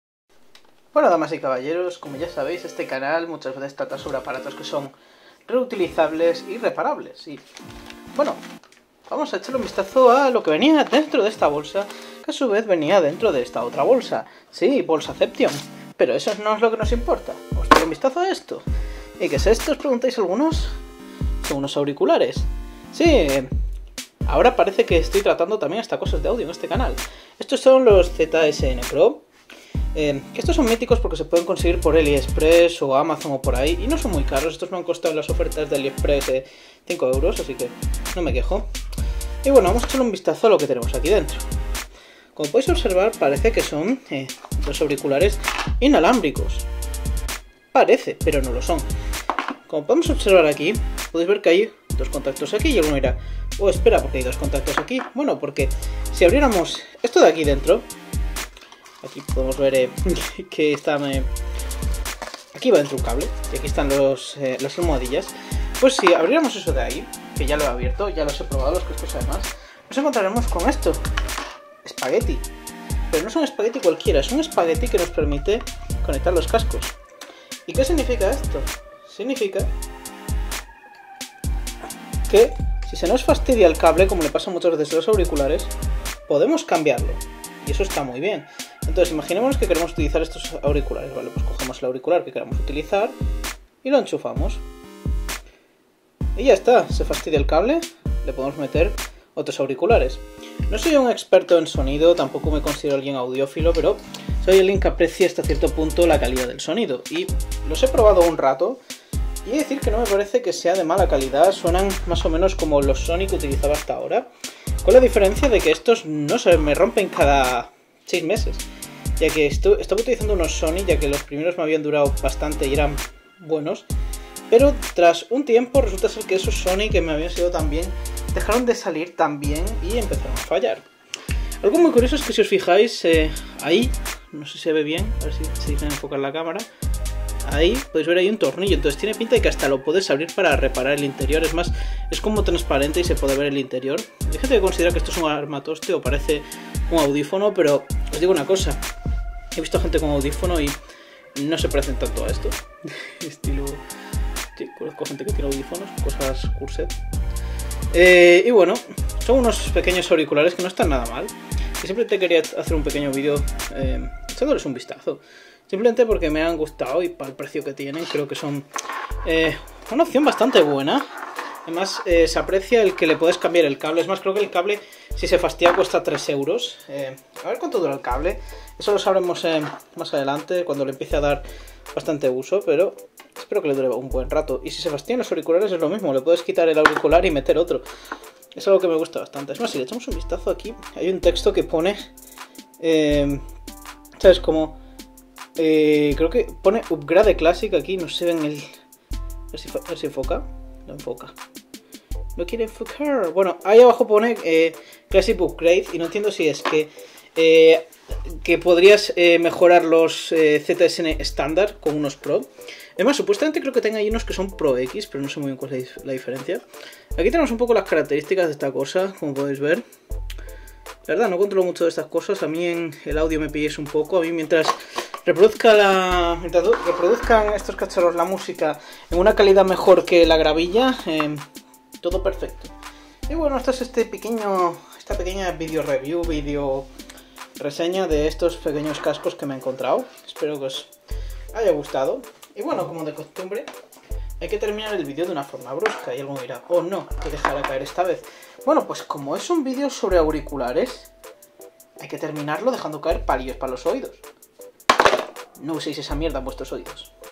ZSN Pro: Los auriculares más BARATOS y REPARABLES del mundo
Hoy os traigo este vídeo sobre estos auriculares que compré ahí atrás en las ofertas por 5 euros para sustituir los Sony que tenía (Ya era el tercer par que fallaba de la misma forma, así que, aunque tenían buen audio, decidí dejar de comparlos). Algo curioso de estos auriculares es que no solo son baratos, también se oyen bien y son bastante reparables.